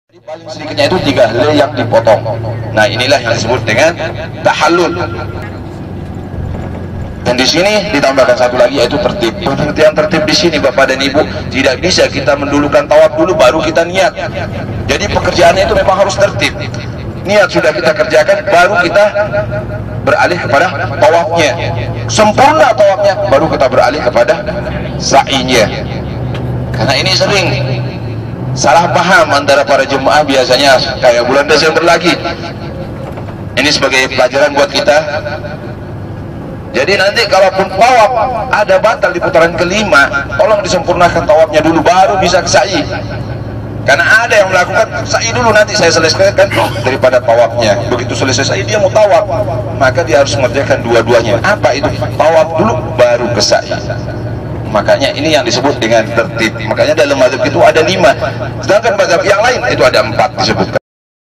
Penjelasan Manasik Umroh dalam Perjalanan ke Makkah
Umroh 2 Juni 2016